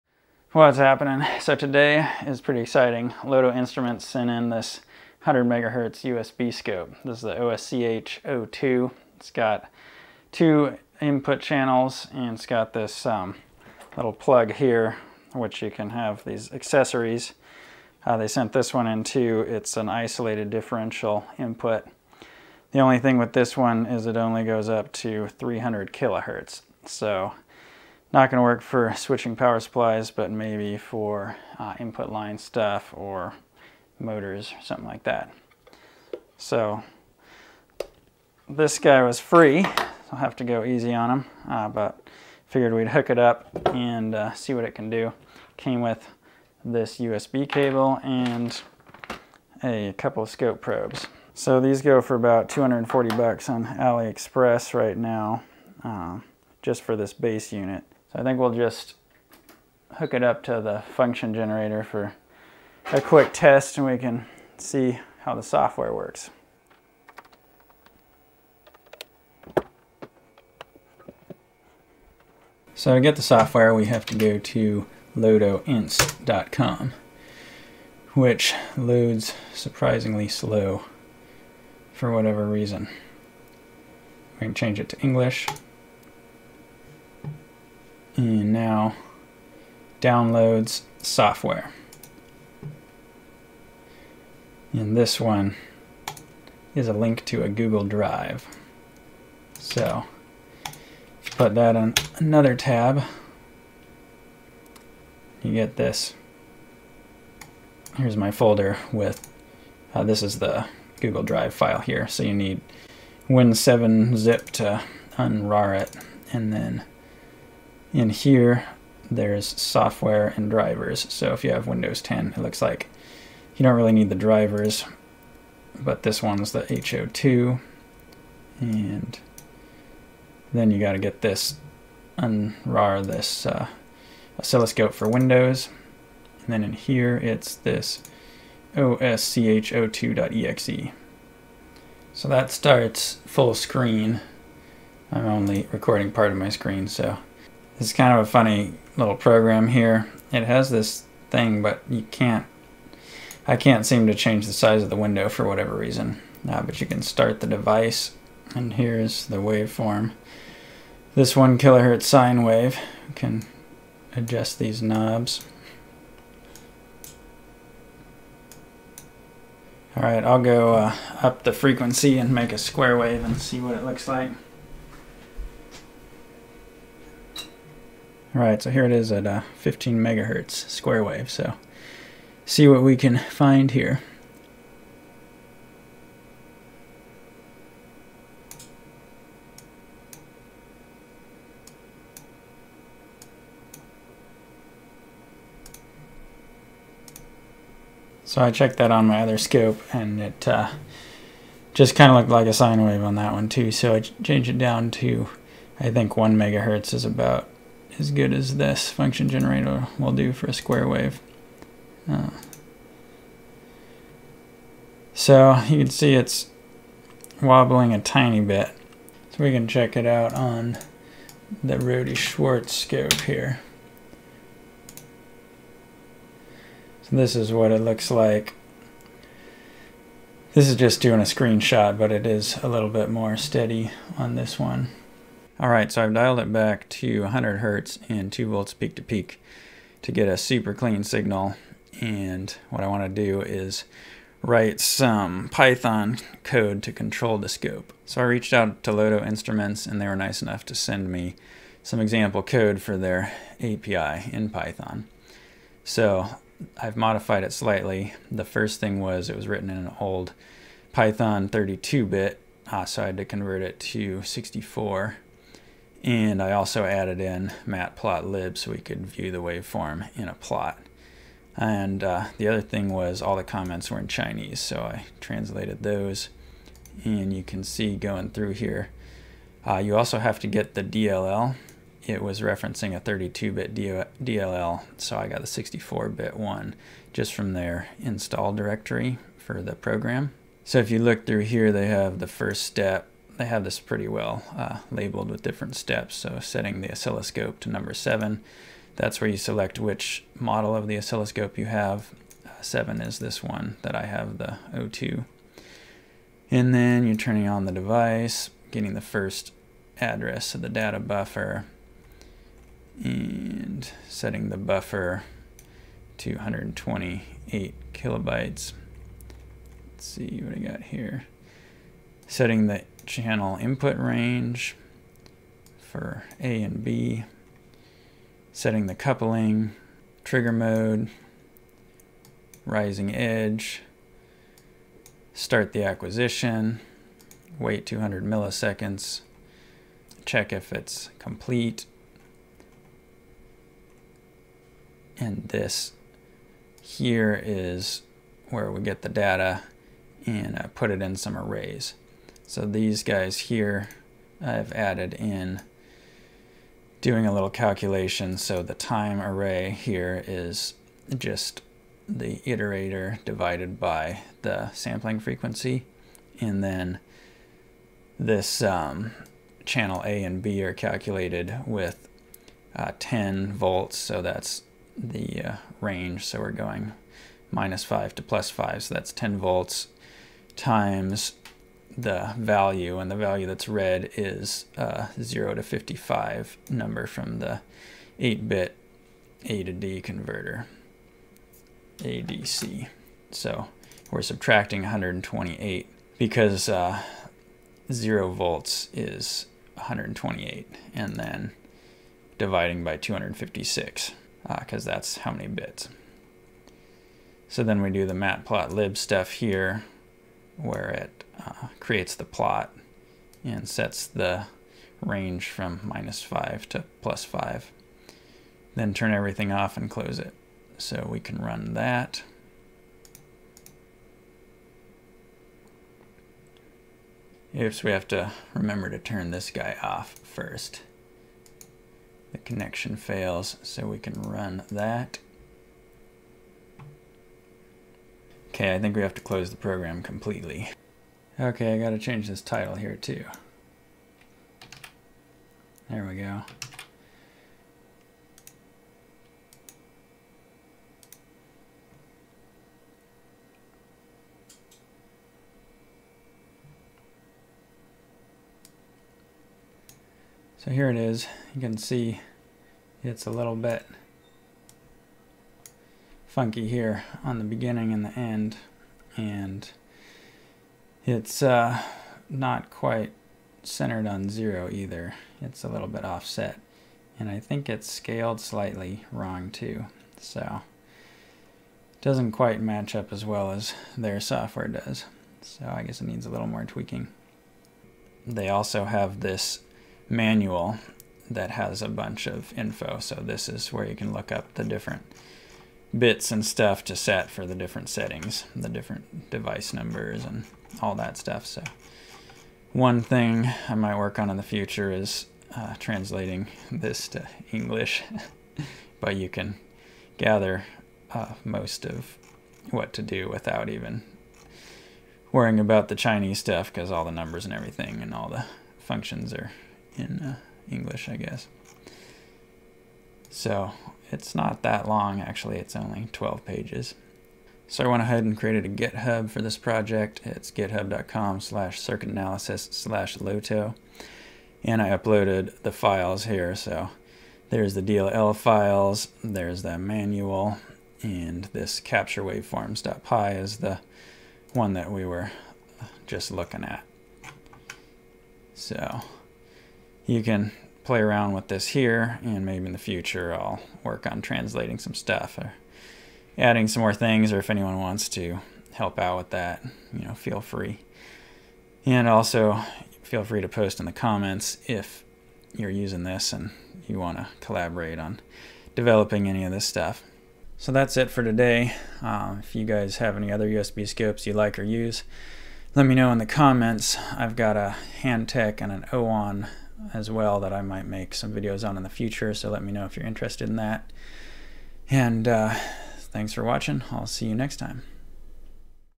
LOTO OSCH02 USB Oscilloscope Review & Scripting Tutorial (Python)
This review/tutorial goes over the setup and programming API for the LOTO OSCH02 USB Oscilloscope.
0:00 Introduction
1:11 Download Software
2:20 View Waveforms
4:07 Python Script
8:05 Documentation
8:47 GitHub Page

Here are some links to resouces discussed in the video:
AliExpress OSCH02
AliExpress LOTO